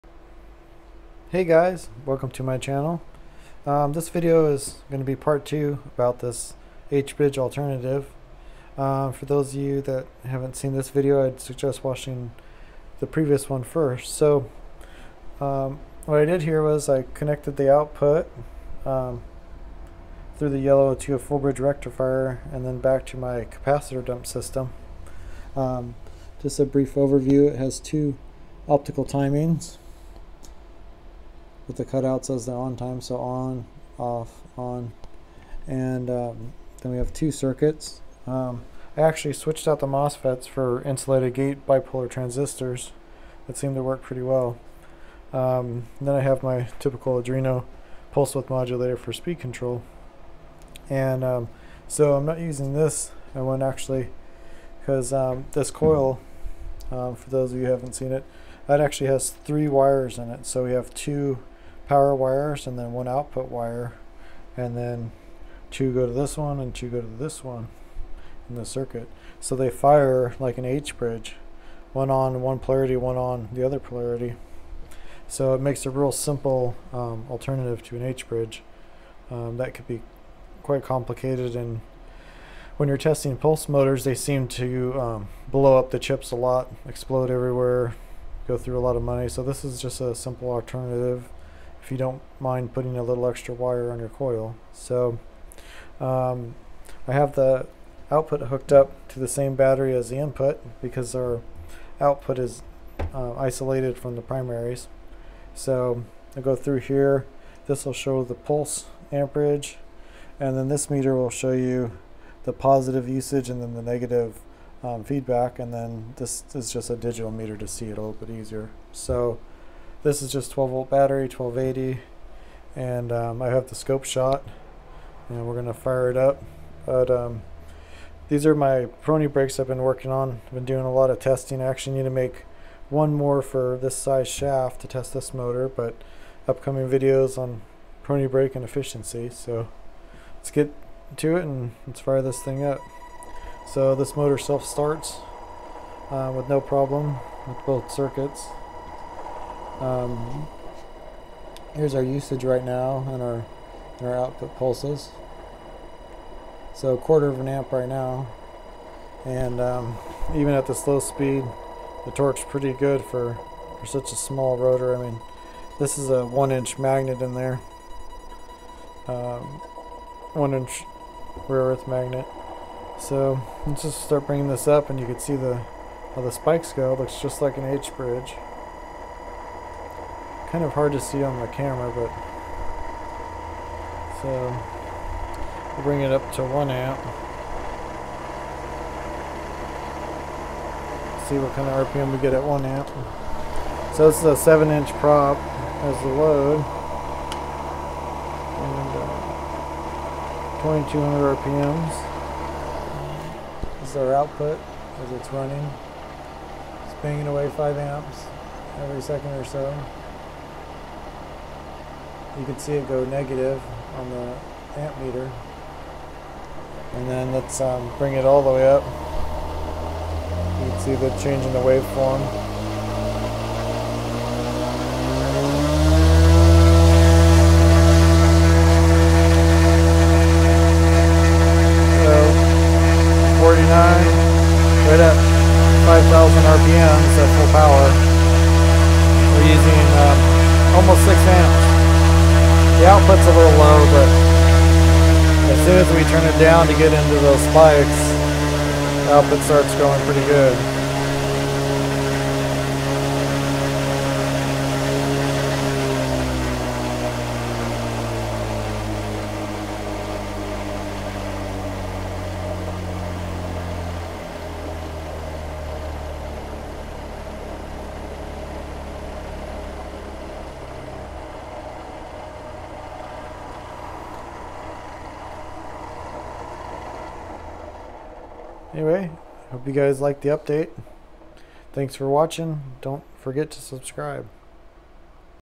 H bridge alternative part 2
12 volt
3 wire coil operated like H bridge both polarity’s with two circuits